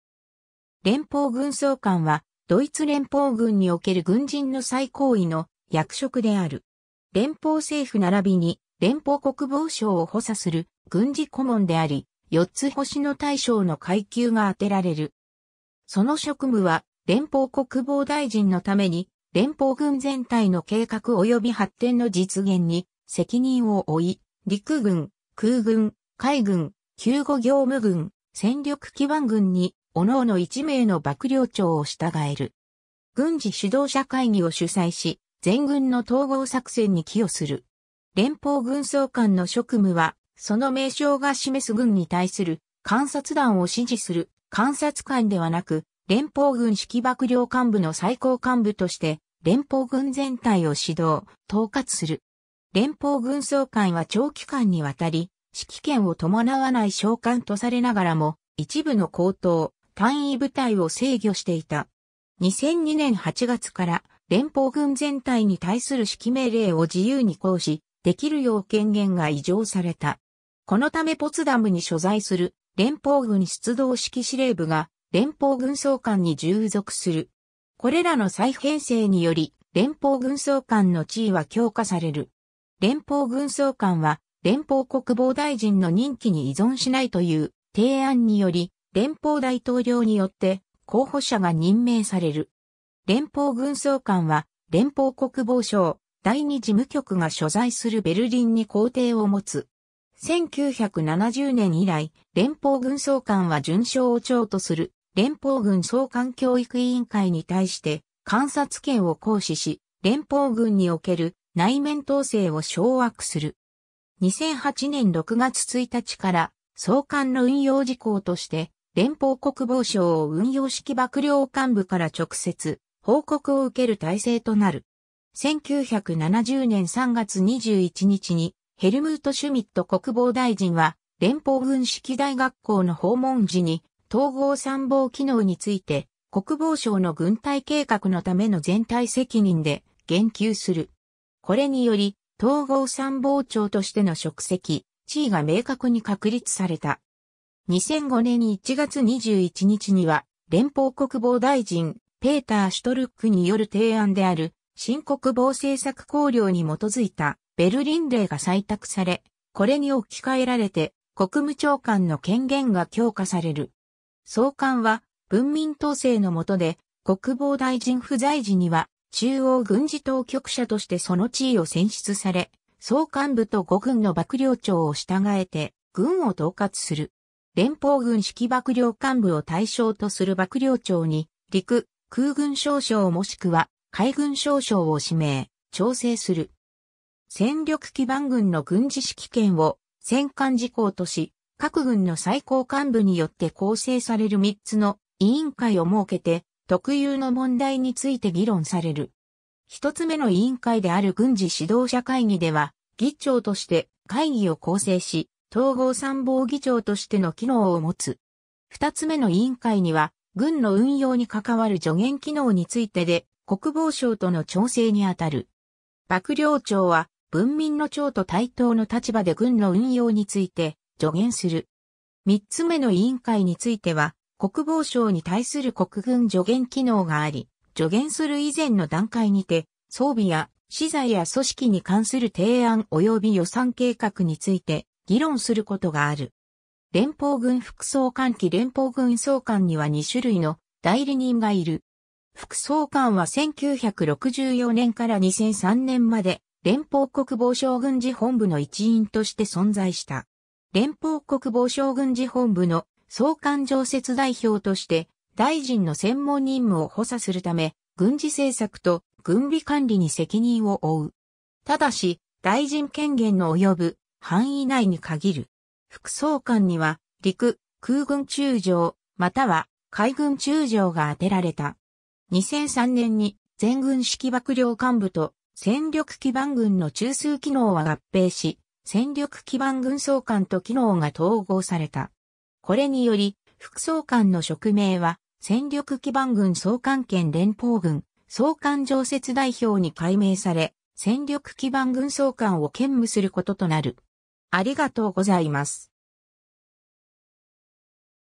連邦軍総監 (ドイツ)
連邦軍総監（れんぽうぐんそうかん ドイツ語: Generalinspekteur der Bundeswehr、略称：GenInspBw）は、ドイツ連邦軍における軍人の最高位の役職である。
連邦政府（連邦首相）ならびに連邦国防省（連邦国防大臣）を補佐する軍事顧問であり、四つ星の大将の階級が充てられる。
 その職務は、連邦国防大臣のために連邦軍全体の計画および発展の実現に責任を負い、陸軍、空軍、海軍、救護業務軍、戦力基盤軍に各々1名の幕僚長を従える。
軍事指導者会議（MFR）を主催し、全軍の統合作戦に寄与する。
 連邦軍総監の職務は、その名称が示す軍に対する監察団を指示する監察官ではなく、連邦軍指揮幕僚監部の最高幹部として連邦軍全体を指導・統括する。
連邦軍総監は長期間にわたり「指揮権を伴わない将官」とされながらも、一部の高等単位部隊を制御していた。
2002年8月から連邦軍全体に対する指揮命令を自由に行使できるよう権限が移譲された。
このためポツダムに所在する連邦軍出動指揮司令部（EinsFüKdoBw）が連邦軍総監に従属する。
これらの再編成により連邦軍総監の地位は強化される。
 連邦軍総監は、連邦国防大臣の任期に依存しないという提案により、連邦大統領によって候補者が任命される。
連邦軍総監は連邦国防省第2事務局が所在するベルリンに公邸を持つ。
 1970年以来、連邦軍総監は准将を長とする連邦軍総監教育委員会に対して監察権を行使し連邦軍における内面統制を掌握する。
 2008年6月1日から、総監の運用事項として連邦国防省運用指揮幕僚監部から直接報告を受ける体制となる。
 1970年3月21日にヘルムート・シュミット国防大臣は連邦軍指揮大学校の訪問時に統合参謀機能について「国防省の軍隊計画のための全体責任（Gesamtverantwortlichen für die Bundeswehrplanung im Verteidigungsministerium）」で言及する。
これにより、統合参謀長としての職責・地位が明確に確立された。
 2005年1月21日には連邦国防大臣ペーター・シュトルックによる提案である新国防政策綱領に基づいたベルリン令が採択され、これに置き換えられて国務長官の権限が強化される。
総監は文民統制の下で、国防大臣不在時には中央軍事当局者としてその地位を選出され、総監部と五軍の幕僚長を従えて軍を統括する。
連邦軍指揮幕僚監部を対象とする幕僚長に陸・空軍少将もしくは海軍少将を指名・調整する。
戦力基盤軍の軍事指揮権を専管事項とし、各軍の最高幹部によって構成される三つの委員会を設けて、特有の問題について議論される。
 一つ目の委員会である軍事指導者会議では、議長として会議を構成し統合参謀議長としての機能を持つ。
二つ目の委員会には軍の運用に関わる助言機能についてで国防省との調整に当たる。
幕僚長は文民の長と対等の立場で軍の運用について助言する。
三つ目の委員会については国防省に対する国軍助言機能があり、助言する以前の段階にて装備や資材や組織に関する提案および予算計画について議論することがある。
 連邦軍副総監旗 連邦軍総監には2種類の代理人がいる。
 副総監は1964年から2003年まで連邦国防省軍事本部の一員として存在した。
連邦国防省軍事本部の総監常設代表（ständiger Vertreter des Generalinspekteurs）として大臣の専門任務を補佐するため、軍事政策と軍備管理に責任を負う。
ただし、大臣権限のおよぶ範囲内に限る。
副総監には陸・空軍中将または海軍中将があてられた。
 2003年に全軍指揮幕僚監部と戦力基盤軍の中枢機能は合併し、戦力基盤軍総監と機能が統合された。
これにより、副総監の職名は戦力基盤軍総監兼連邦軍総監常設代表（Stellvertreter des Generalinspekteurs der Bundeswehr und Inspekteur der Streitkräftebasis）に改名され、戦力基盤軍総監を兼務する事となる。